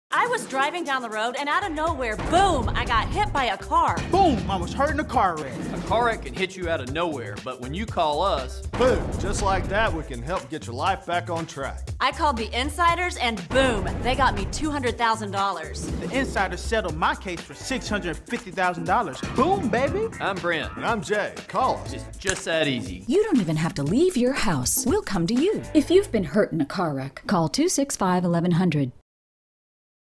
Car Accidents Happen Fast, Call the Insiders!
You can be driving along and BOOM!

Have you or a loved one been hurt in an accident in the Chattanooga or Knoxville, TN area? Call the Insiders! Brent Burks and Jay Kennamer at the McMahan Law Firm are experienced personal injury lawyers dedicated to helping injured victims and their families get the money they truly deserve.

You don't even have to leave your house, we'll come to you.

Have questions? Ask the Insiders!